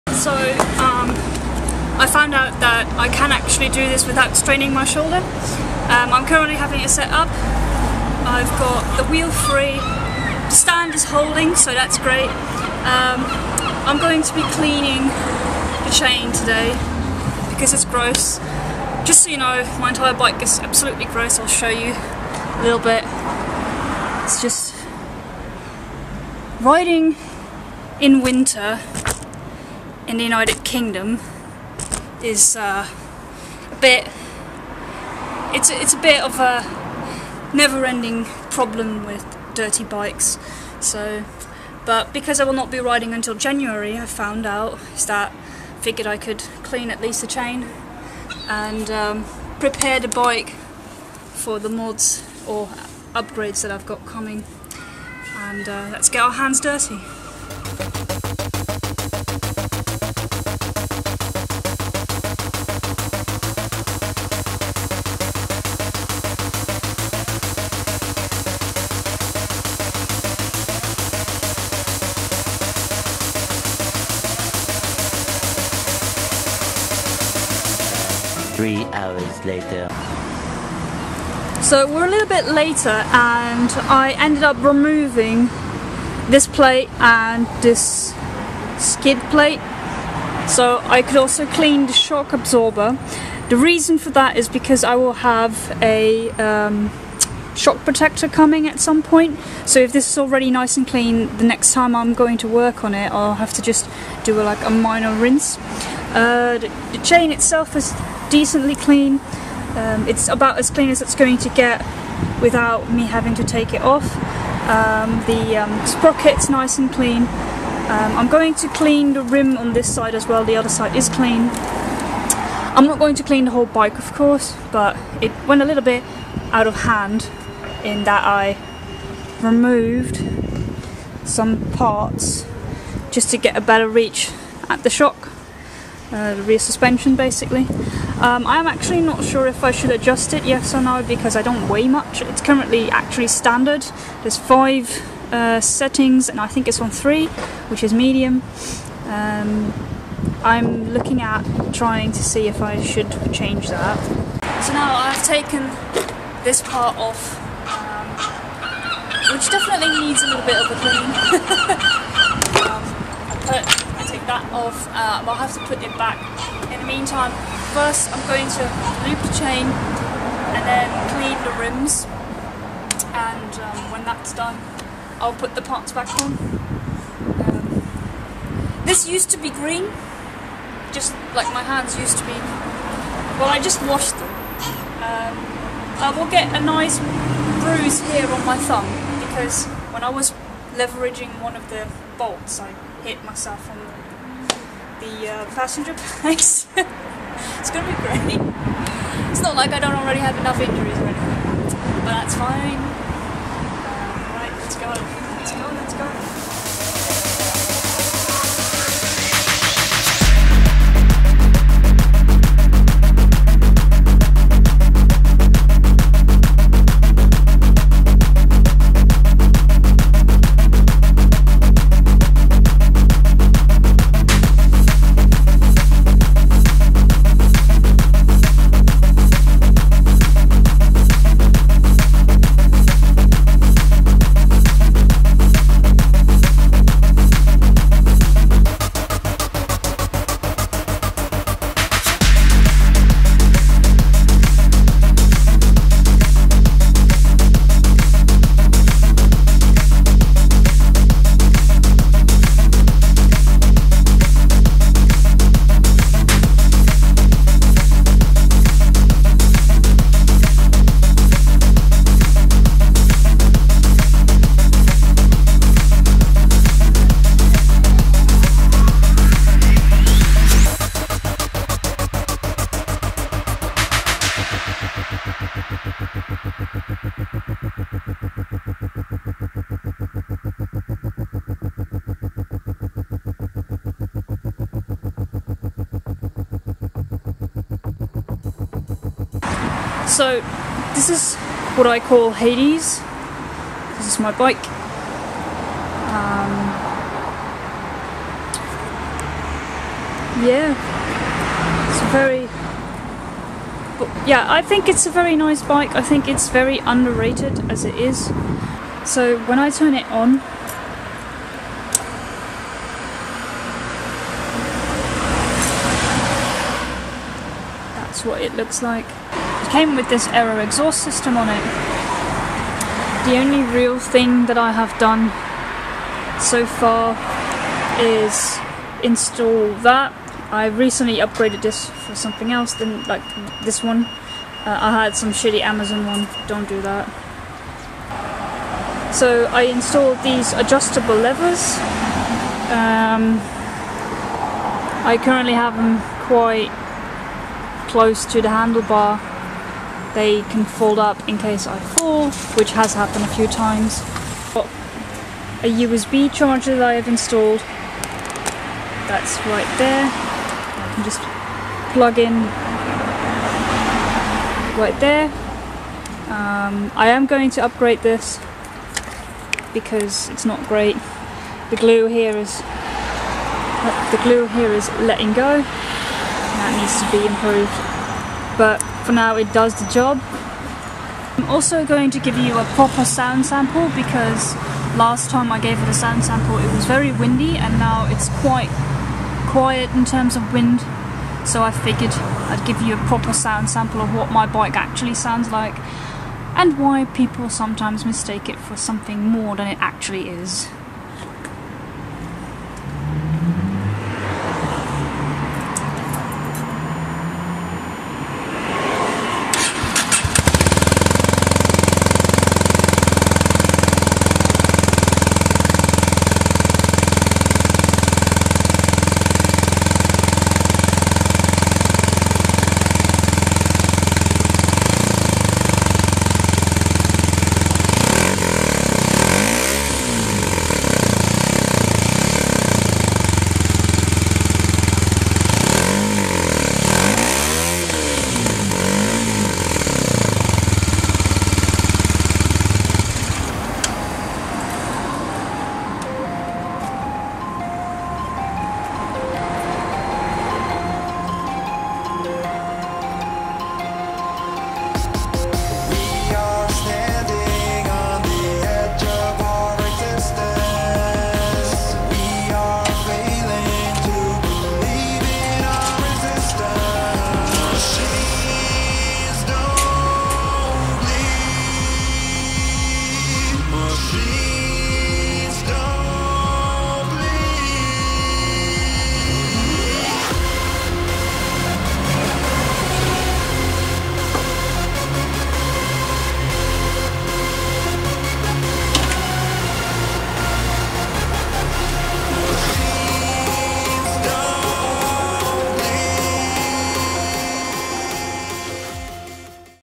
Kawasaki Ninja 125 || Chain maintenance & rear-shock cleaning
Hi everyone,
As you can see I found a way to do this without straining my shoulder. It started with just wanting to clean the chain and lube it, but it quickly became a more intense cleaning session, in preparation for one of the upgrades coming.

Bike: 2019 Kawasaki Ninja 125cc Performance Edition
Helmet: Scorpion EXO-1400 Air
Gloves: Furygan Ares & Icon Twenty Niner ladies
Boots: Alpinestars Stella
Trousers: Furygan Ladies Veloche / Rev'it Outback 3 Ladies
Jacket: Furygan Ladies Luxio / Ace Motorsport (unknown model, charity shop find).
Armour: D3O back, shoulders, elbows, hips, knees